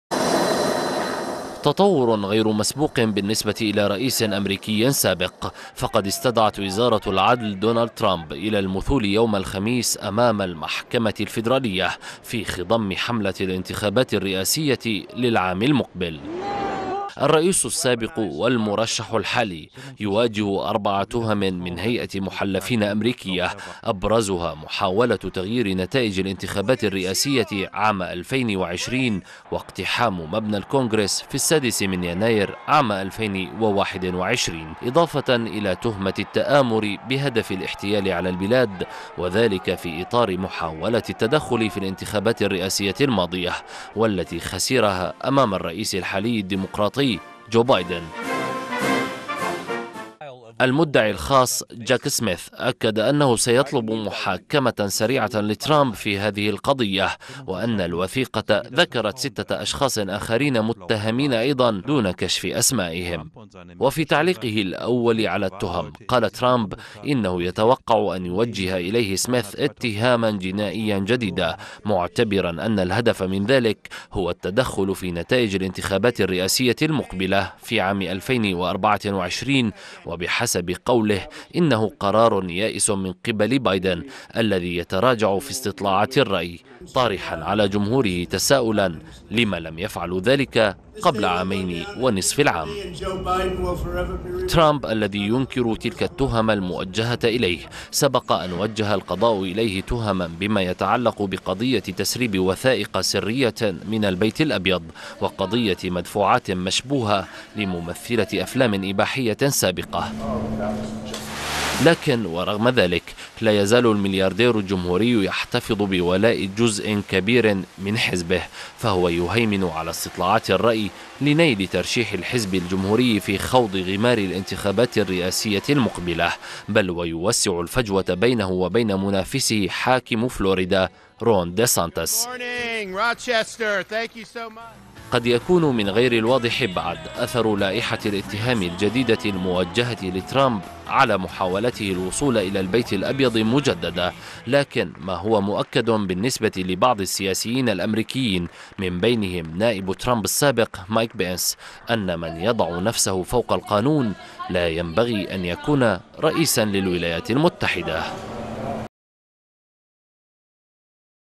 ترمب يواجه 4 اتهامات بينها محاولة تغيير نتيجة الانتخابات الرئاسية لعام 2020
وجّهت هيئة محلفين أمريكية لائحة اتهام إلى الرئيس السابق دونالد ترامب تتضمن 4 تهم جديدة، أبرزها "محاولة تغيير نتيجة الانتخابات الرئاسية 2020".